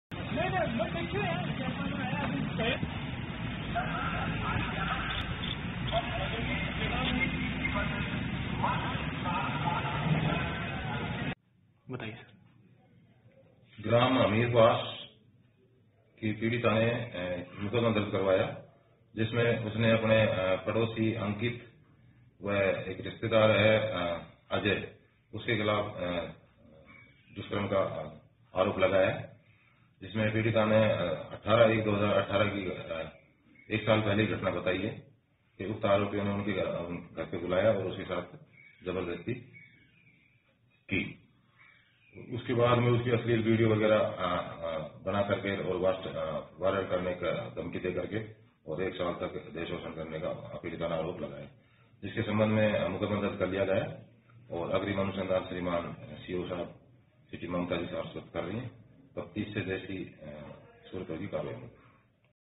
झुंझुनूं: सदर थाना इलाके में युवती के साथ सामूहिक दुष्कर्म कर ब्लैकमेलिंग, मामला दर्ज
झुंझुनूं. जिले के हमीरवास इलाके में एक युवती से सामूहिक दुष्कर्म कर ब्लैकमेलिंग का मामला सामने आया है. पीड़िता ने सदर थाने में मामला दर्ज कराया है. पुलिस पीड़िता का मेडिकल करवाकर मामले की जांच कर रही है.